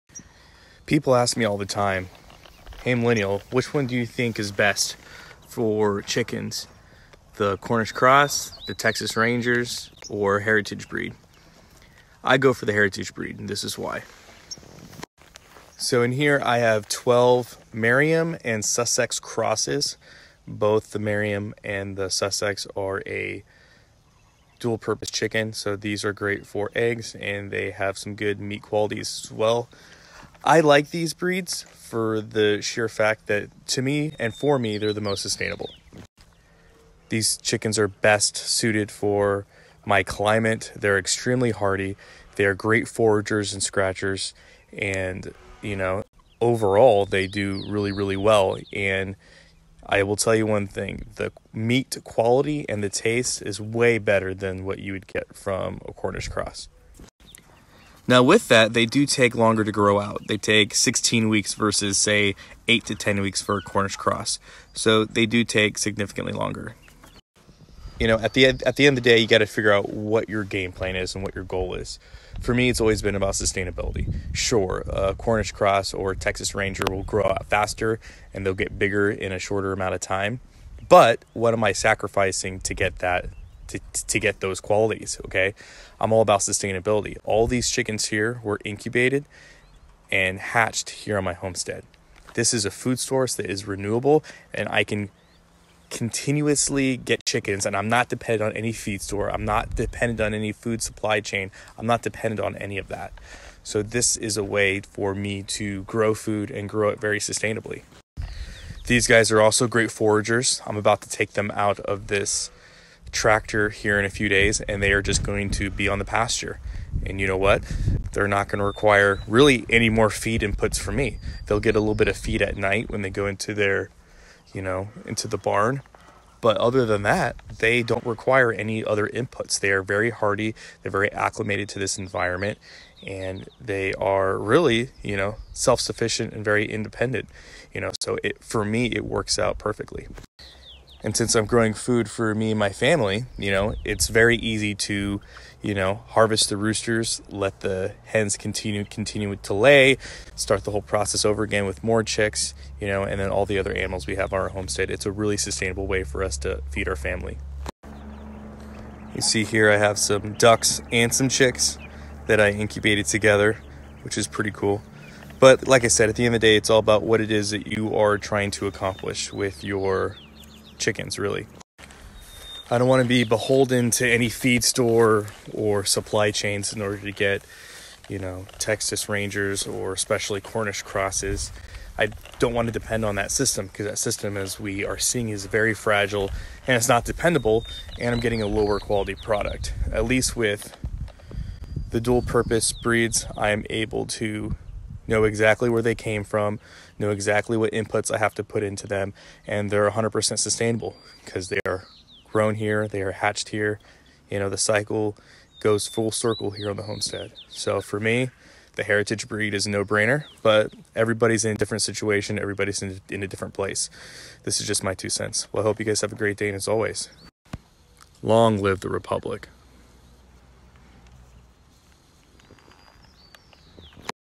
Meat Chickens Vs. Dual Purpose Chickens
In this video I explain why I like Dual purpose Chickens over Meat Chickens.

Pros:
1) More Sustainable
2) Better suited my Environment and climate
3) Excellent Foragers (requires less feed)
4) Better quality and tasting meat
5) Can be used for both meat and eggs
6) More Hardy
7) Zero Dependance on feed stores, big animal agriculture or the supply chain for chickens.

Cons:
1) Smaller then meat birds
2) Take longer to grow out